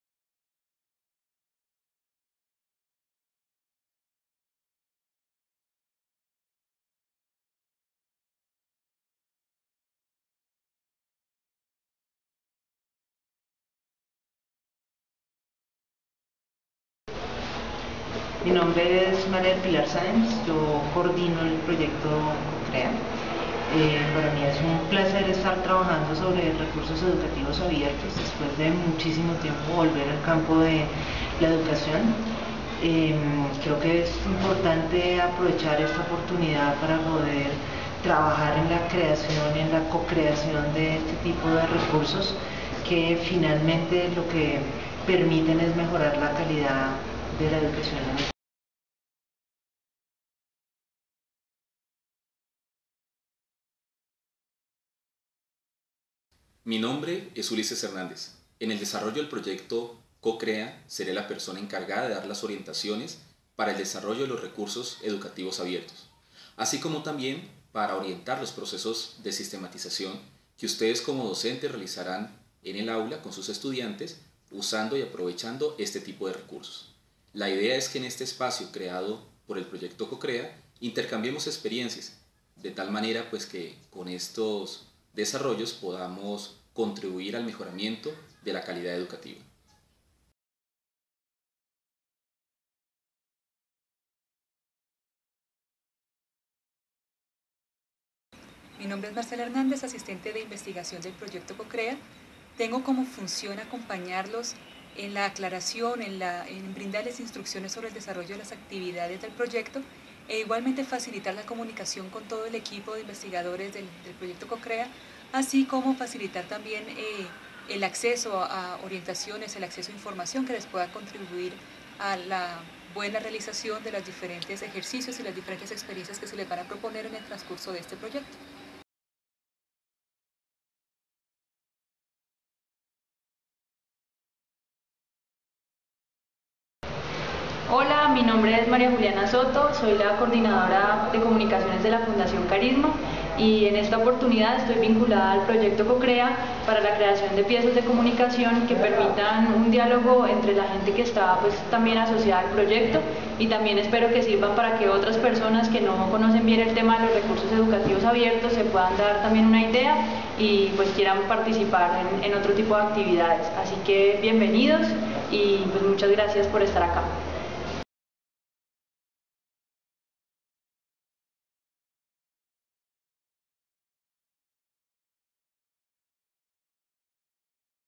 Seminario virtual de iniciación del proyecto co-KREA
co-KREA es un proyecto de co-creación colaborativa de Recursos Educativos Abiertos (REA) entre formadores de docentes y docentes en el suroccidente colombiano: un caso de estudio desde la perspectiva: investigación-acción participativa.